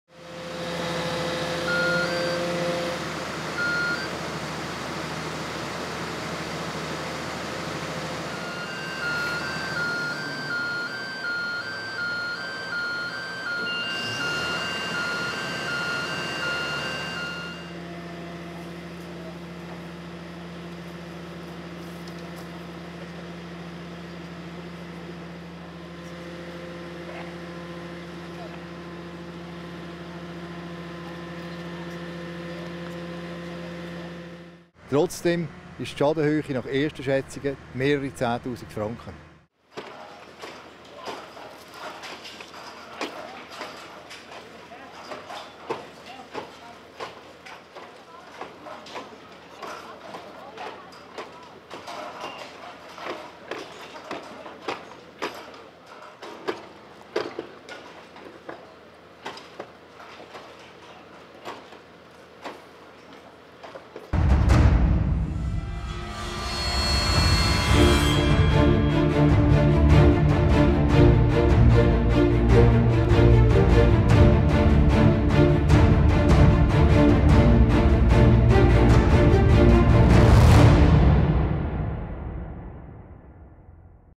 Glattfelden ZH: Brand verursacht Sachschaden
Bei einem Brand in einer Schreinerei ist am Mittwochabend ein Sachschaden von mehreren zehntausend Franken entstanden. Anwohner bemerkten kurz vor 23:00 Uhr Feuer in der Schreinerei und alarmierten die Rettungskräfte. Die Feuerwehren waren schnell vor Ort und brachten den Brand unter Kontrolle. Verletzt wurde niemand. Die Brandursache ist derzeit nicht bekannt. Sie wird von Spezialisten der Brandermittlungsgruppe der Kantonspolizei Zürich untersucht.